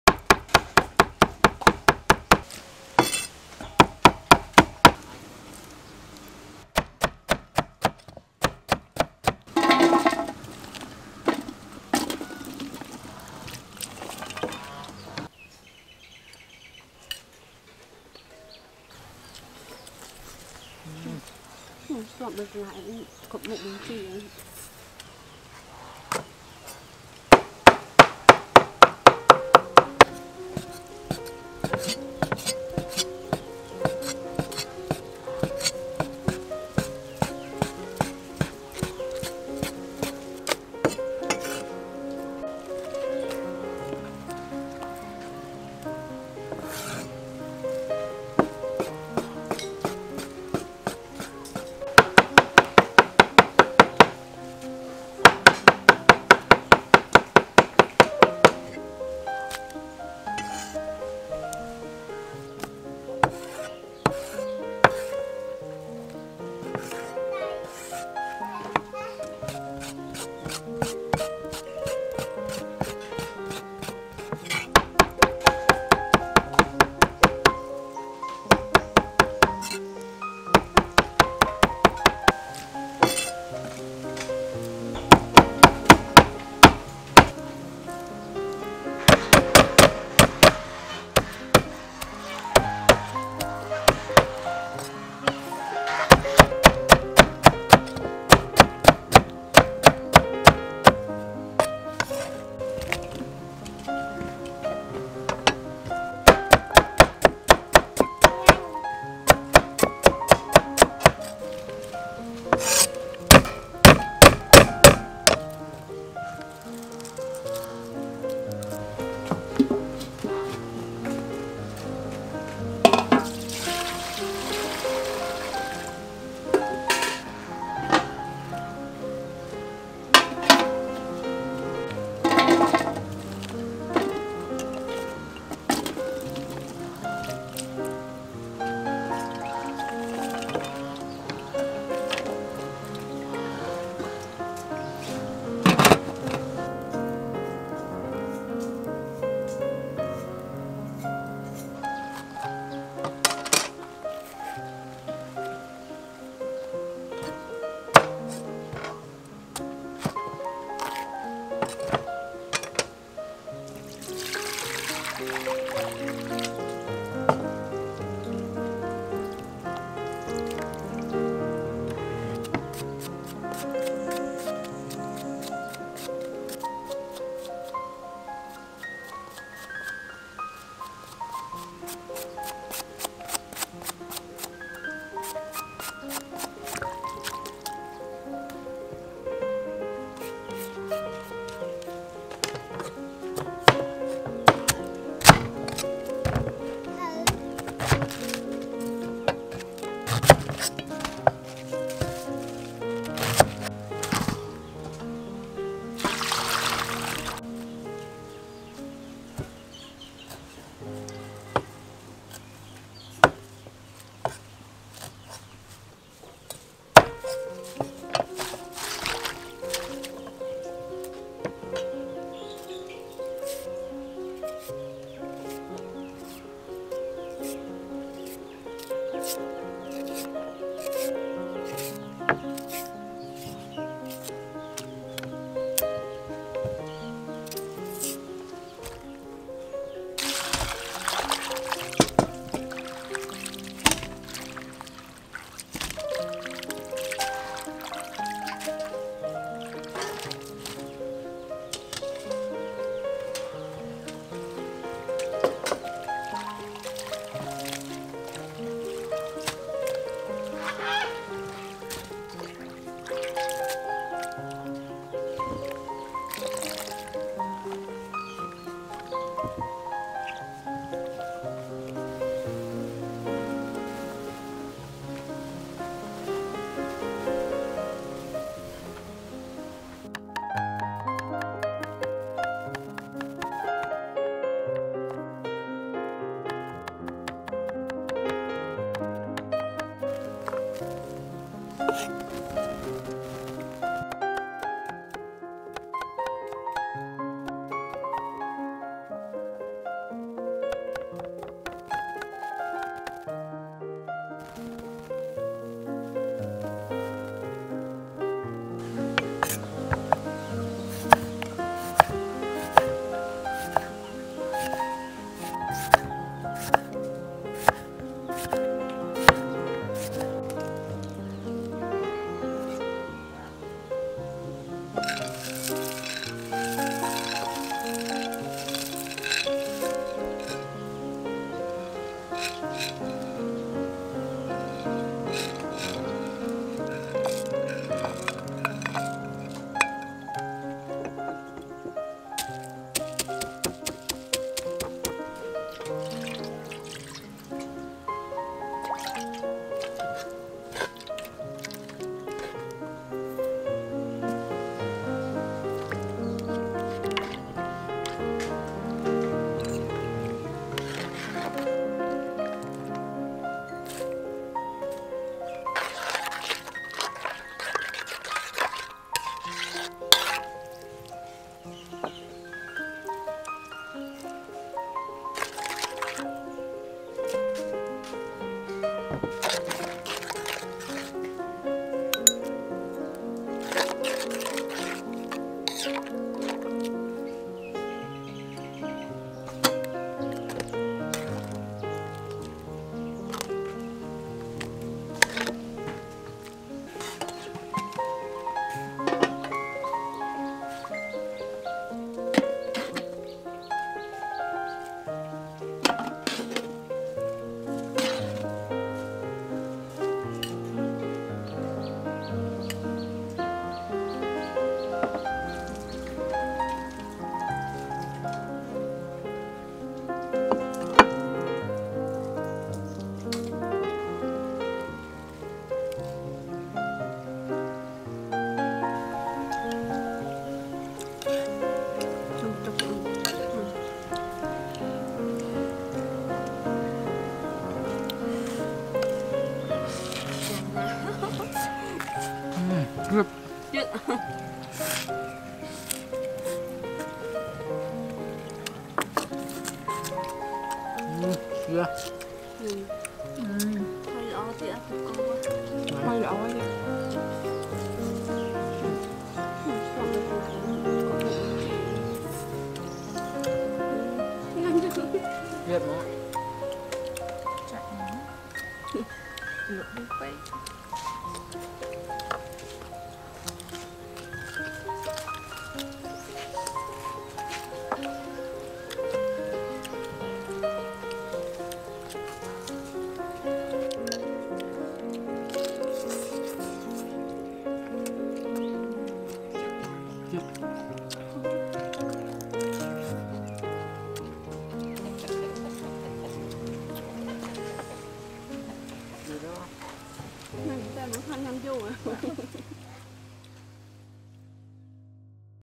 STEAM Beef Intestine and enjoy with family | Beef Intestine Cook and Eat | Cooking with Sros
Wow! Beef intestine steam with fresh ingredient and enjoy with my brother and sister.
Fish past sauce that we eat in this video is so amazing that you might like.